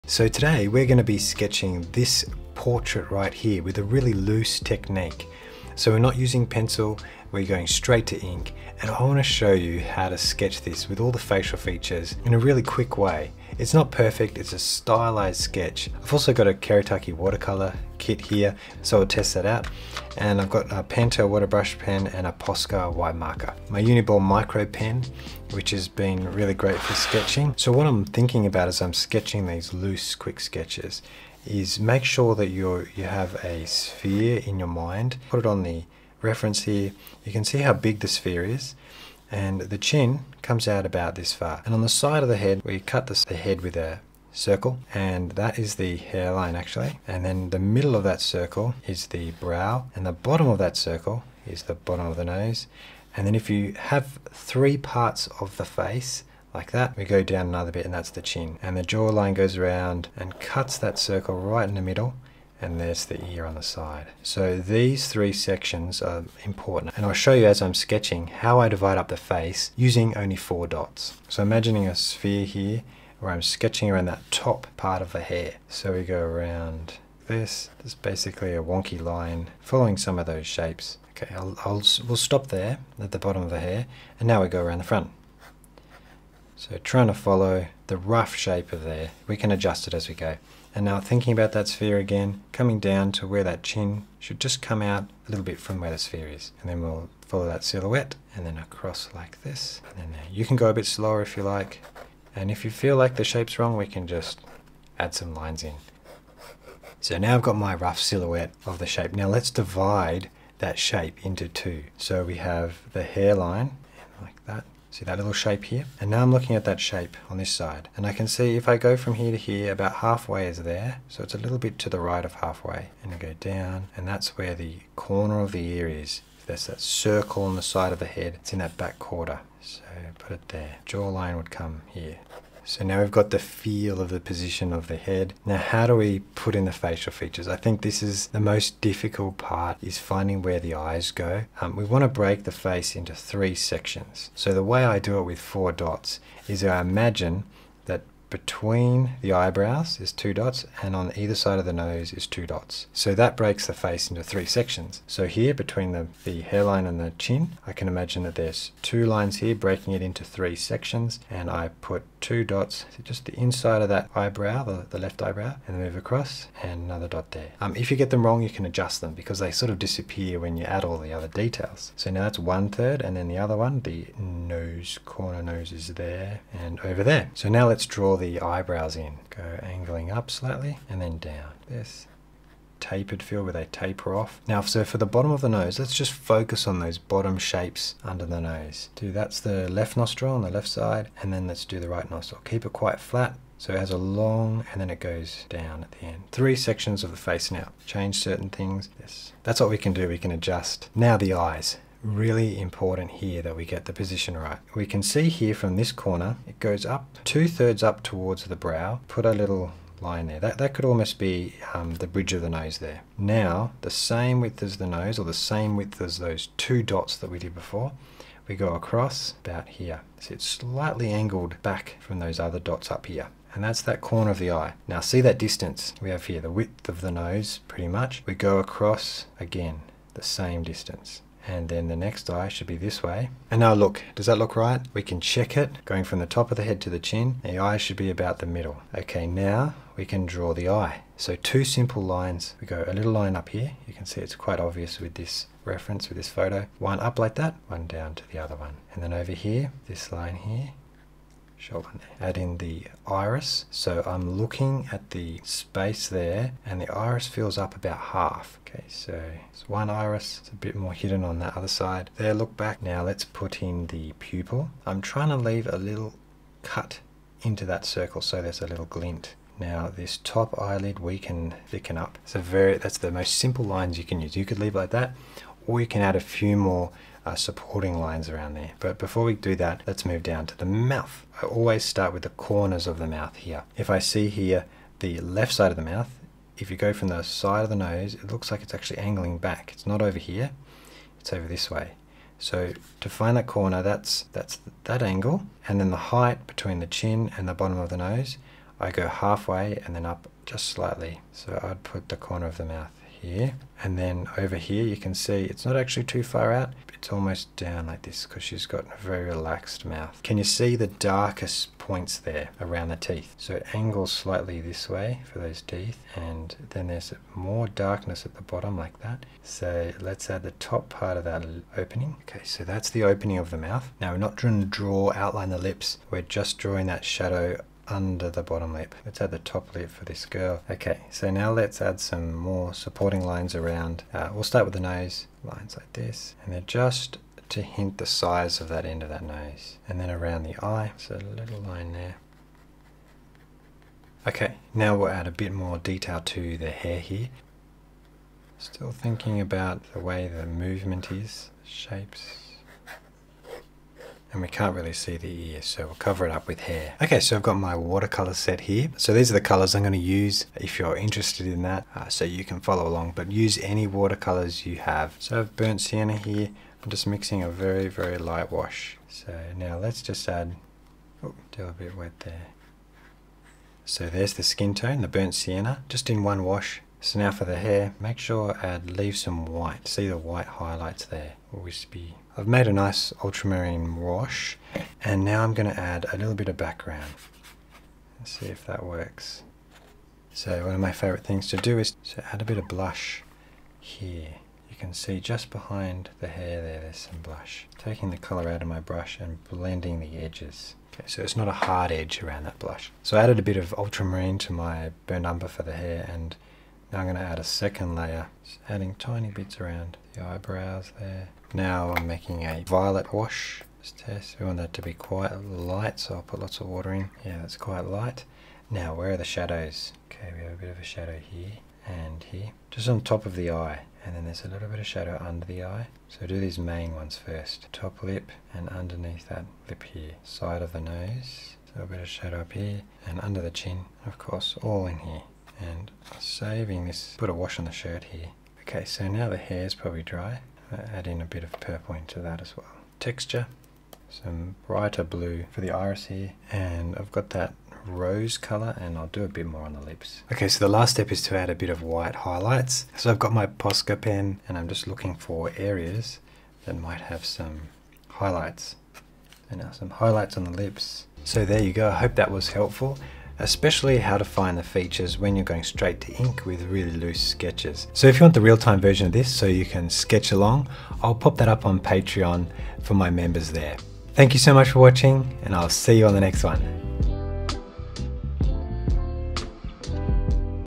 Simple INK PEN drawing technique - Improve your portraits!
Follow this simple sketching tutorial on how to draw a portrait when going straight to ink.

(In-depth tutorials, templates, references & feedback)

✍🏼SKETCHING MATERIALS USED (or closest I could find on Amazon)
Etchr Lab A6 sketchbook
Uni-Ball Vision (Micro 0.5mm)
Kuretake 14 watercolor set
Pentel Water Brush (L)
Posca Fine Marker White